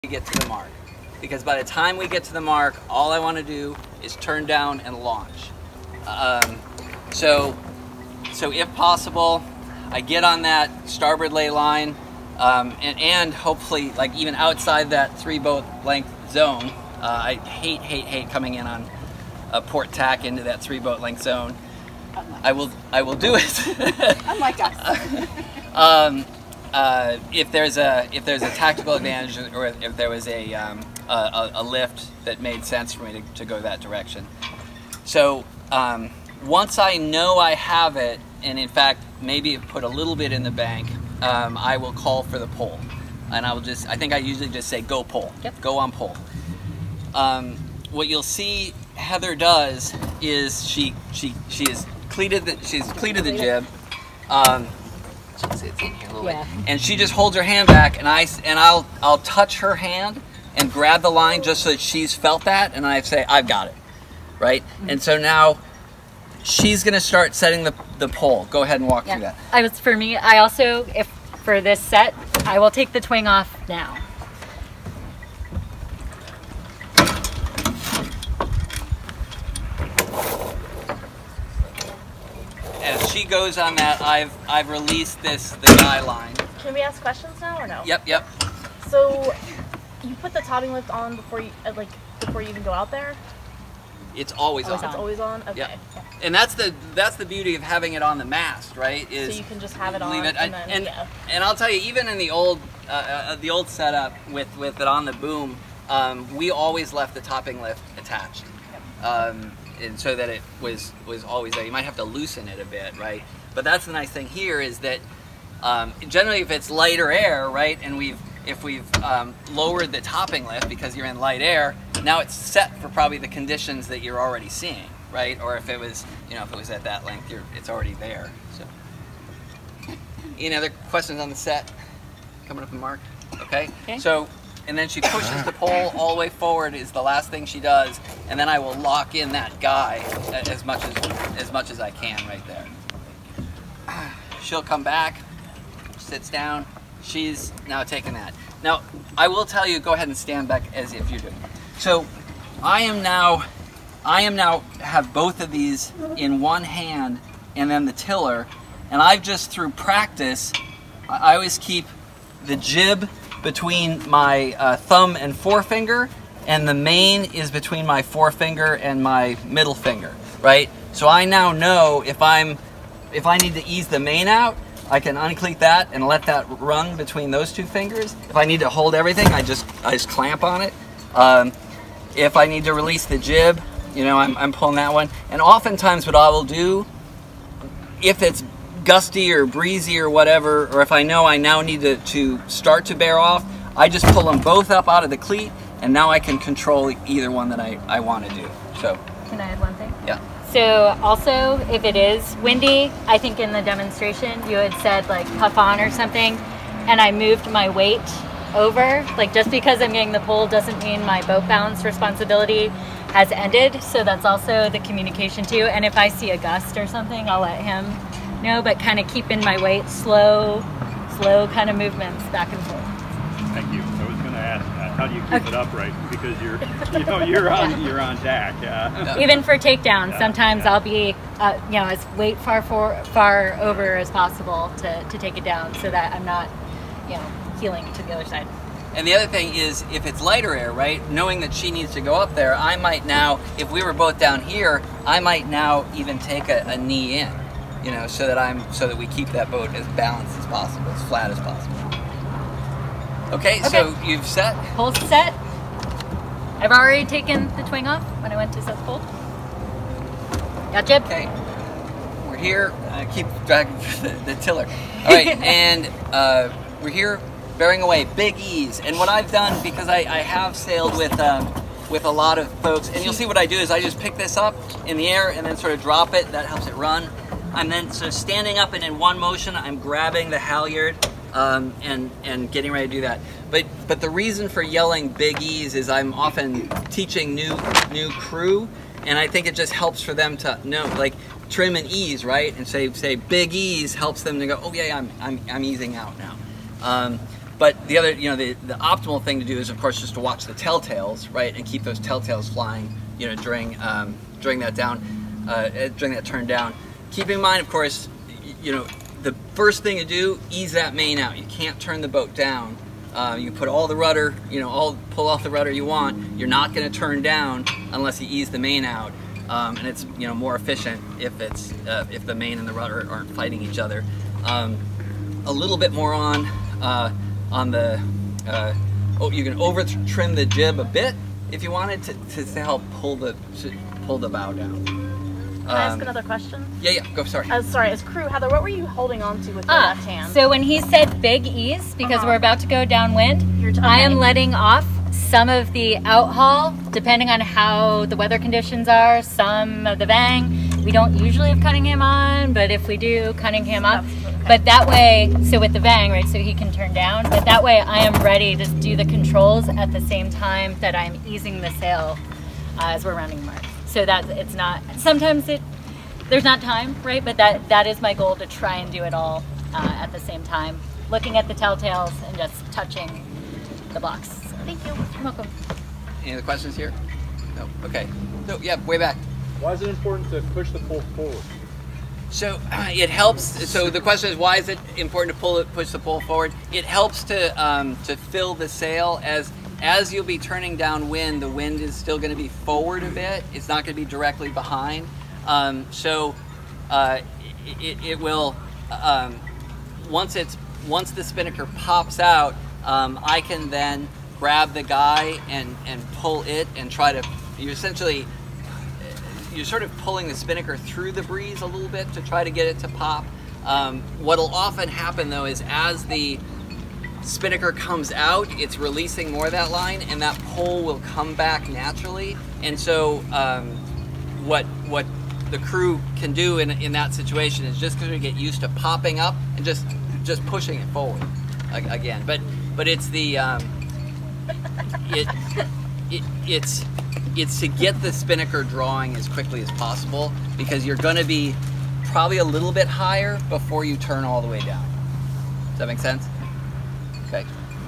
Spinny Flying Getting to the mark and setting the pole 1 of 5
Rigging tips,   line handling,  crew communication, team roles & responsibilities, and boat balance & control